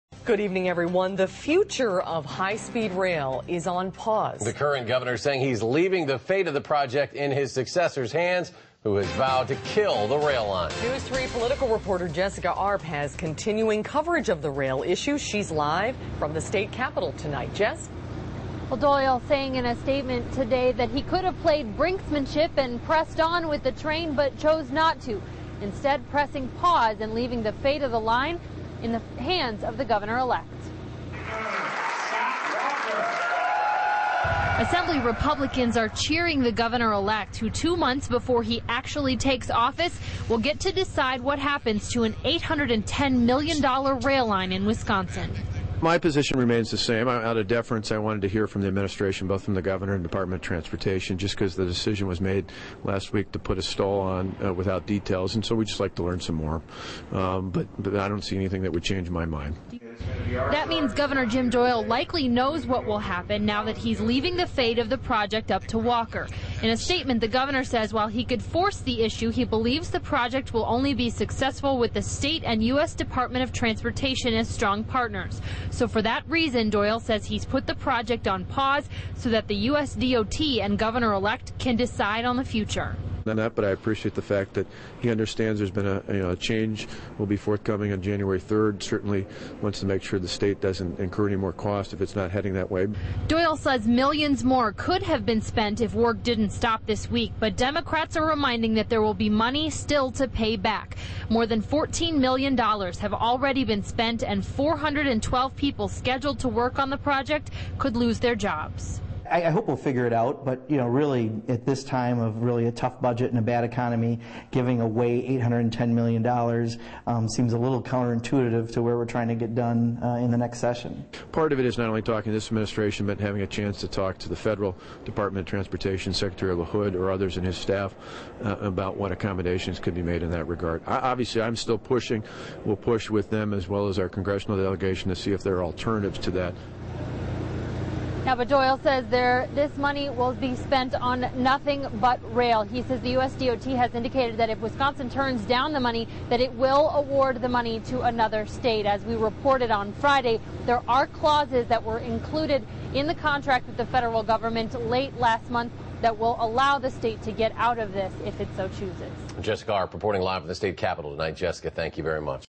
Doyle Says He'll Leave Train Project To Walker
Outgoing Democratic Gov. Jim Doyle said he is leaving the future of
Wisconsin's high-speed train line to his Republican successor who has
vowed to kill the project. Jessica Arp reports